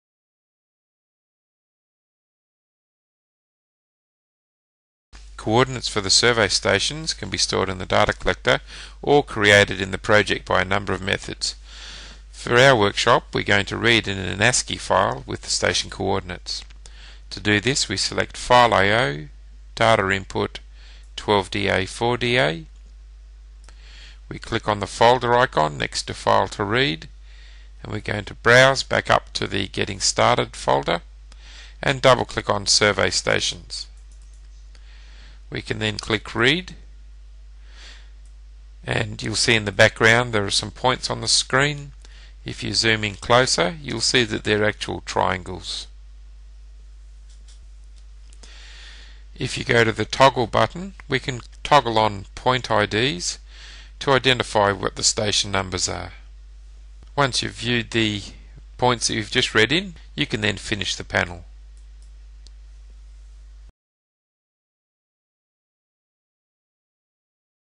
12d Model - Detail Survey - Control Co-ordinate Entry (Getting Started for Surveying)
The Getting Started for Surveying video series is designed to give new 12d Model users a basic understanding of the Survey-based features of 12d Model software.

This video provides an overview of control co-ordinate entry in 12d Model.